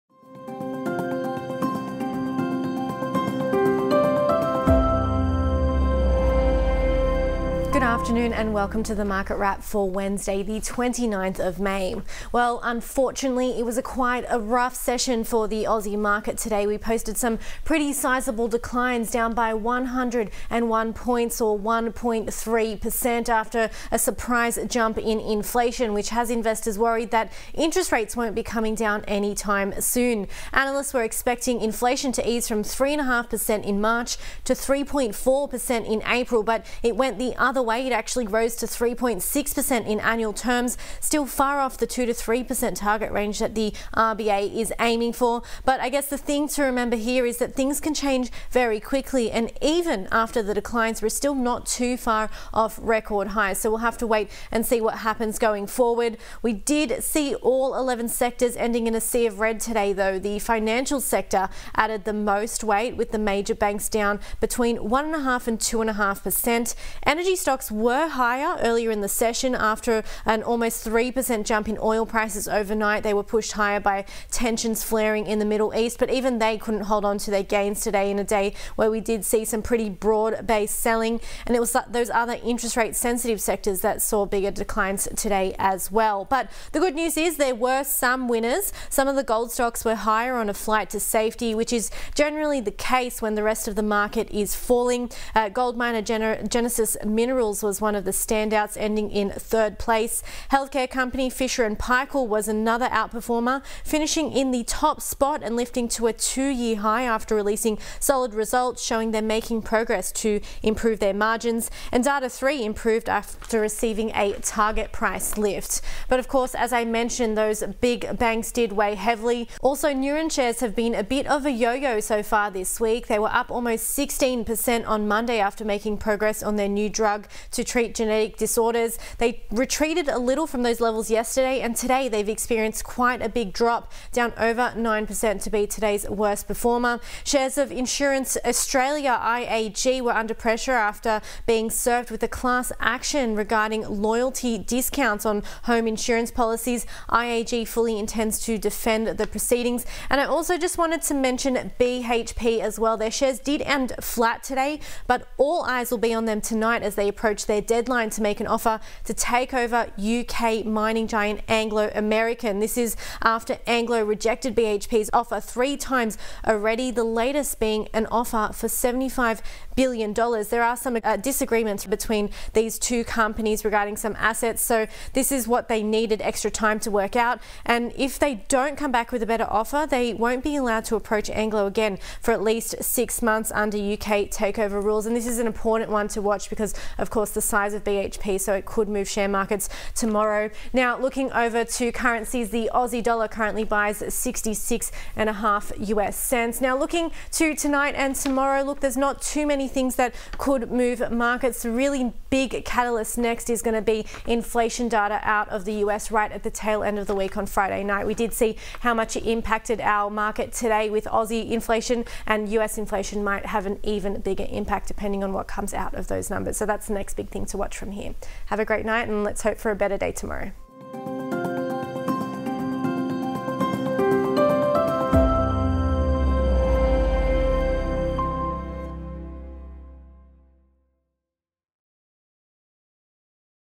Market Close 29 May 24: A rough session
The Aussie market experienced a rough session on Wednesday after inflation data surprisingly jumped from 3.5% in March to 3.6% in April. This has investors on edge that interest rates will remain where they are for an extended period of time.